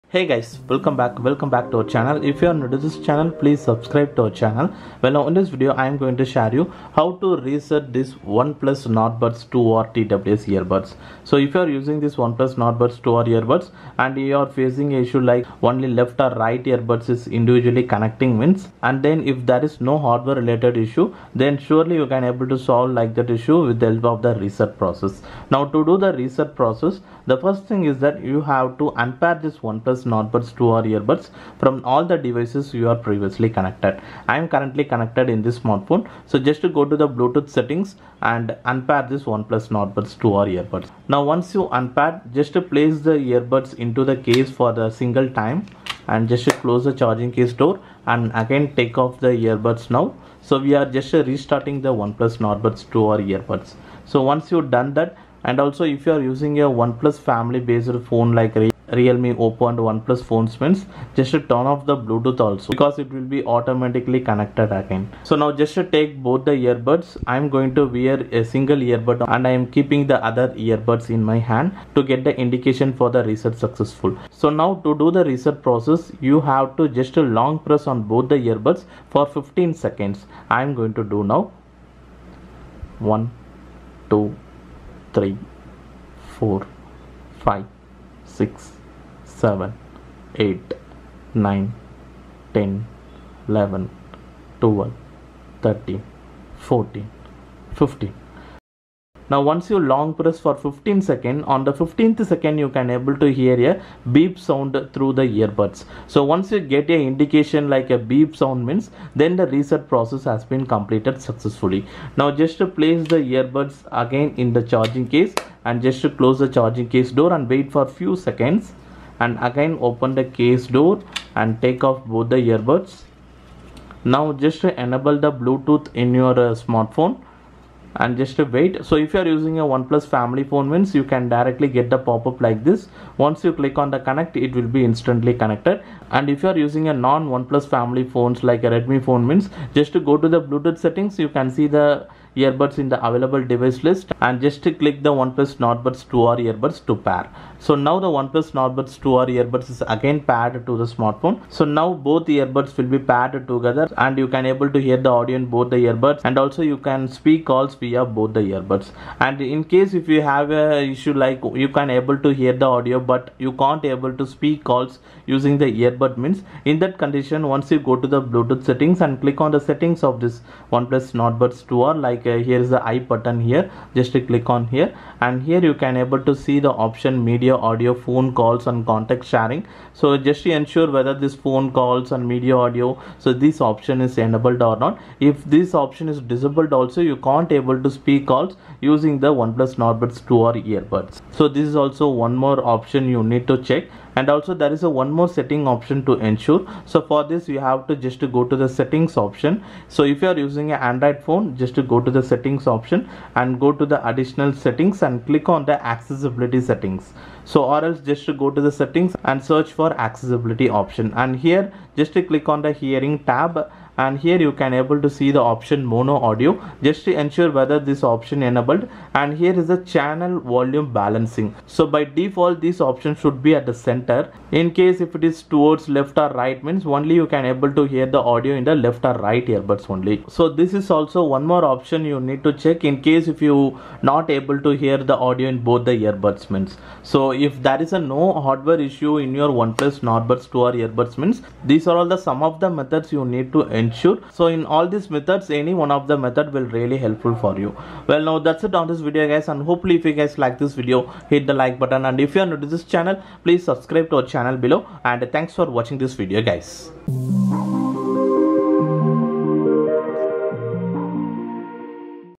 How to reset OnePlus Nord Buds 2r Earbuds - OnePlus Earbuds one side not Pairing/Working problem?
Product Listings:
OnePlus Nord Buds 2r

In this video, let me share the method of How to reset OnePlus Nord Buds 2r earbuds so that if your OnePlus Nord earbuds on any one side not pairing or working then you easily connect by doing the reset process...